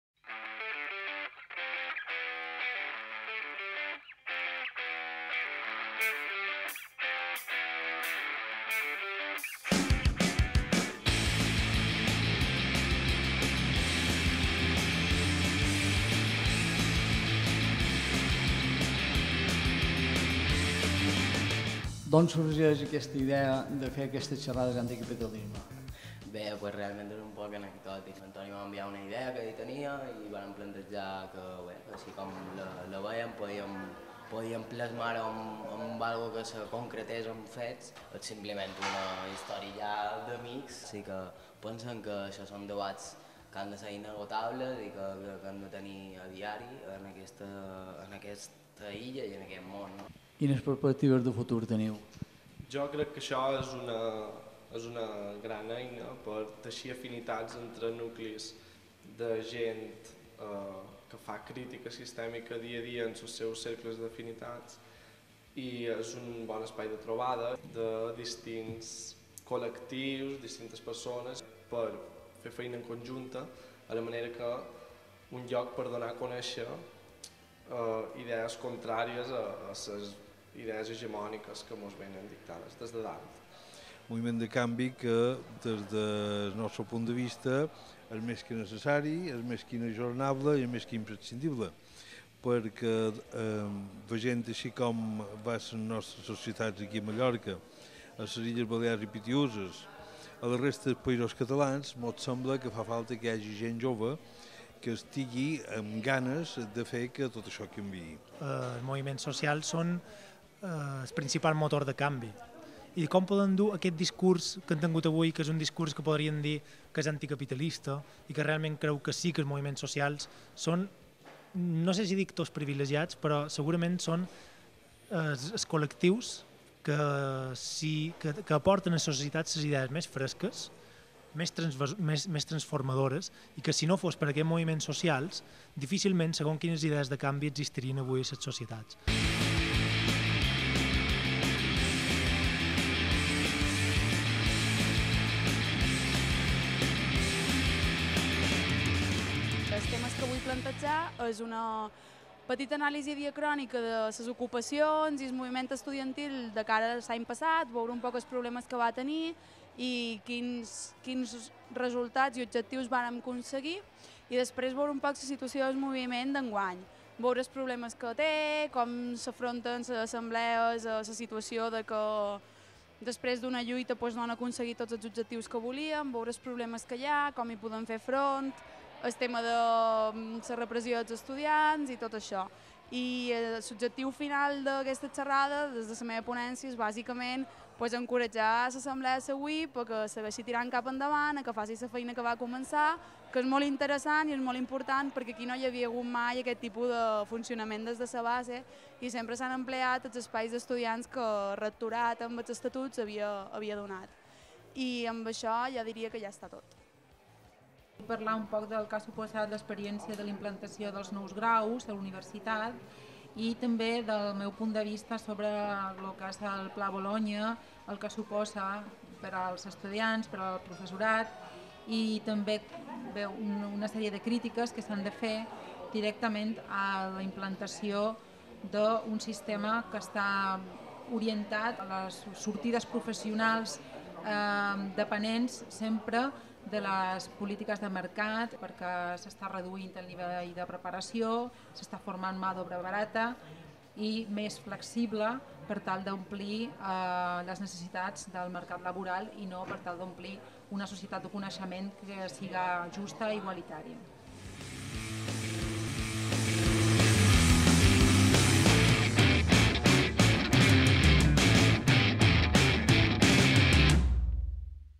Xerrades Anticapitalistes. Palma, març de 2010
Cil Buele, Arnau Matas, Neus Roca i Laura Camargo ens expliquen per damunt el contingut de les seves intervencions a les Xerrades Anticapitalistes celebrades el mes de març de 2010 a Ca'n Alcover i a l'aula de Graus de l'edifici Ramon Llull de la UIB. Aquesta és una iniciativa dels Estudiants Anticapitalistes de la UIB i la organització va córrer a càrrec de Toni Pizà i Borja Rosa